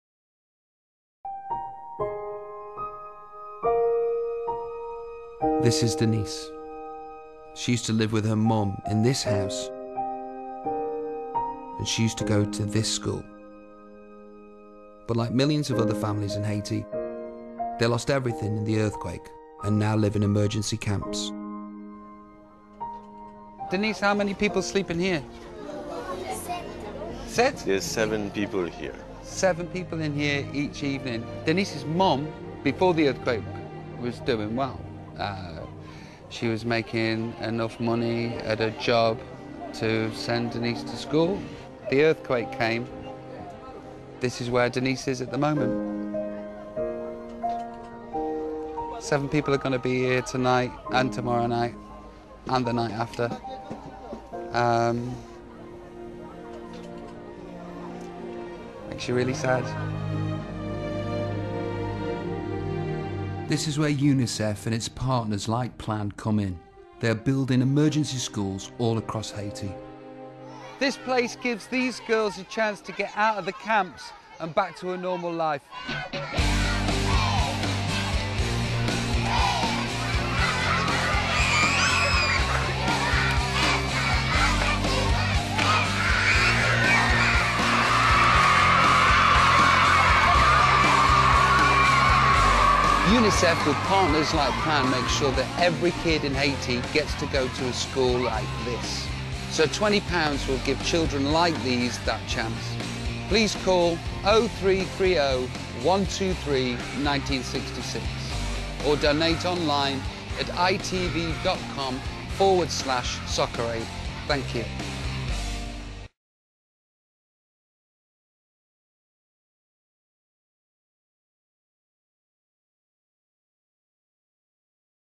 Robbie Williams's Soccer Aid appeal video from Haiti part 3
Robbie Williams visits Haiti to find out how money raised by Soccer Aid is helping UNICEF help children affected by the devastating earthquake of January 12 2010.